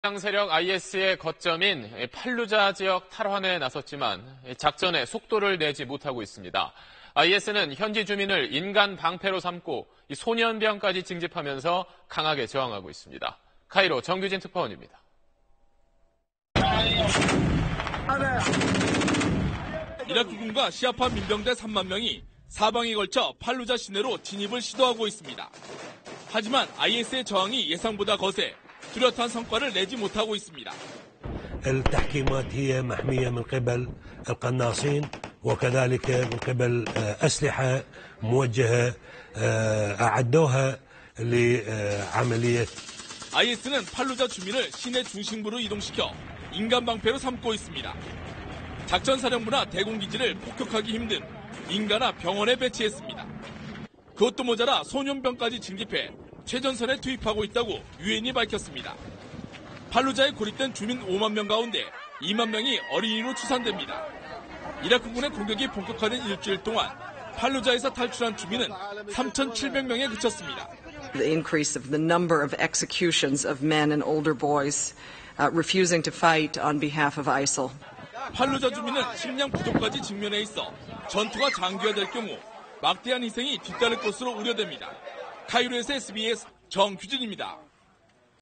철벽방어 나선 IS…"소년병 징집해 최전선 투입" / SBS
이라크군이 이슬람 무장세력 IS의 거점인 팔루자 지역 탈환에 나섰지만, 작전에 속도를 내지 못하고 있습니다. IS는 현지 주민을 인간 방패로 삼고 소년병까지 징집하면서 강하게 저항하고 있습니다.